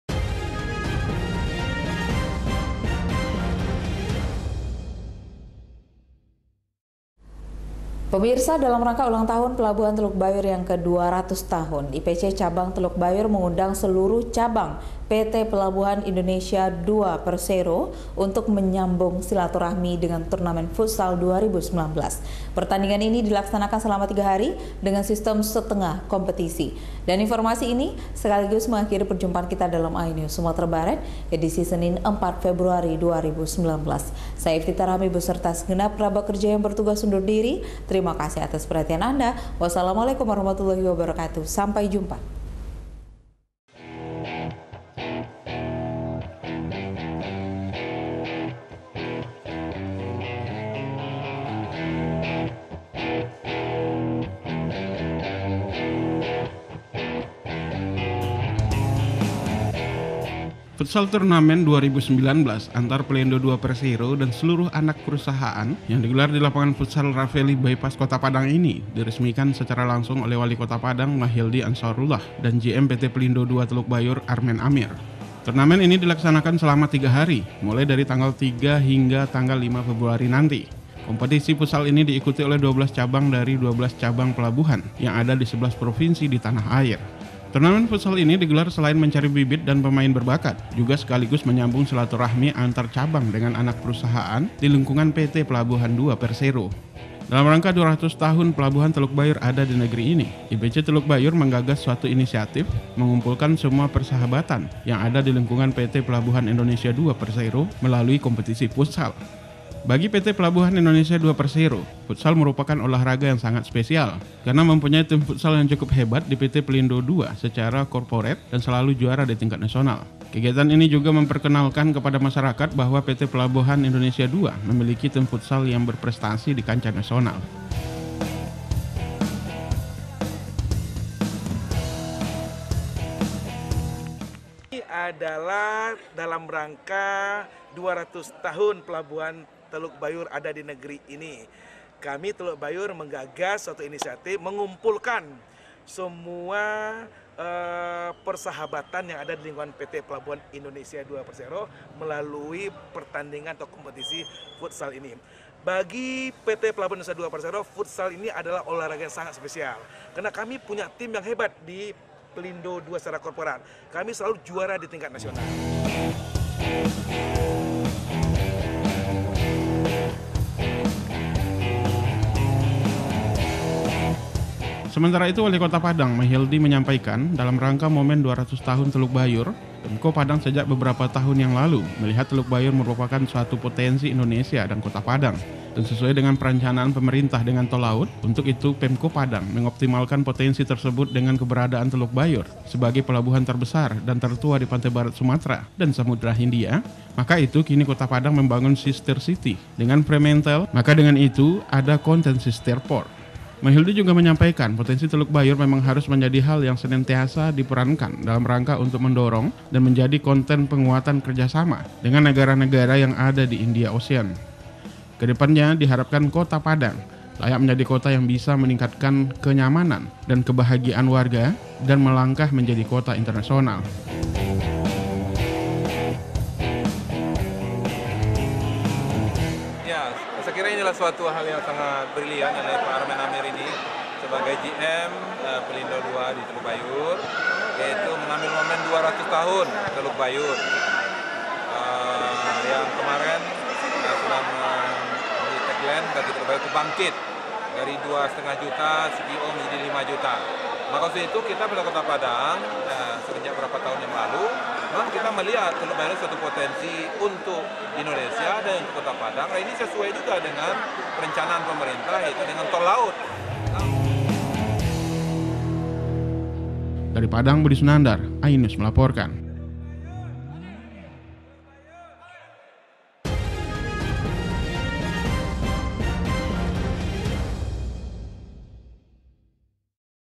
Turnamen Futsal Pelindo II
Dalam rangka ulang tahun Pelabuhan Teluk Bayur ke 200 tahun, IPC Teluk Bayur mengundang seluruh cabang PT. Pelabuhan Indonesia II Persero, untuk menyambung silaturahmi dengan turnamen futsal 2019, di lapangan Rafely Futsal di Kota Padang.